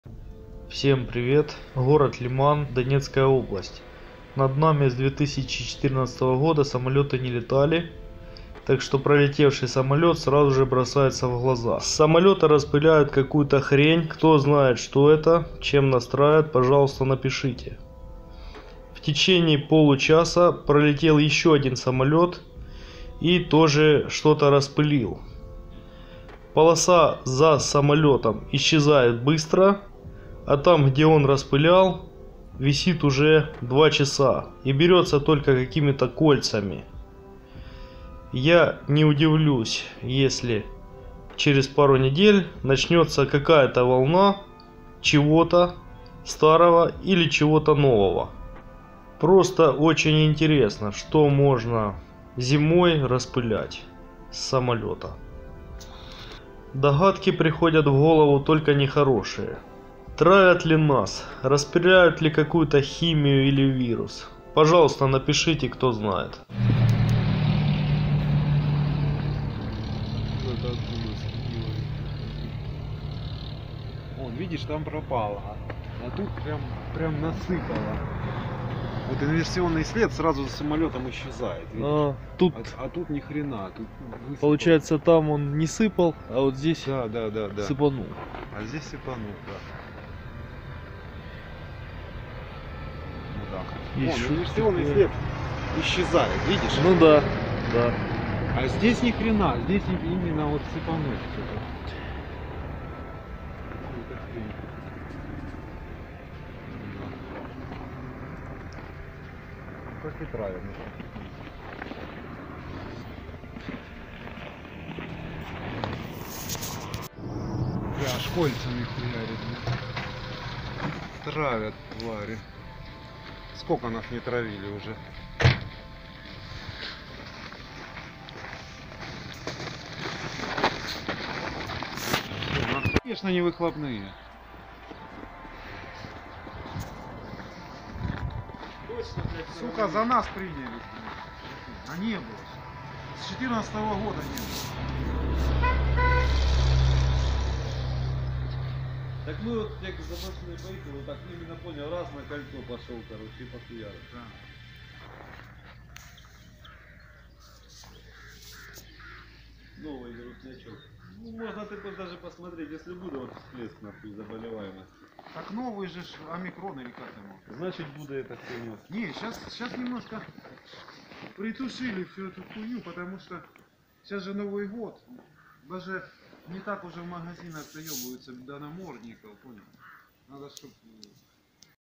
Что распыляют с самолётов? Чем травят?
Над Лиманом распылили неизвестное вещество с самолёта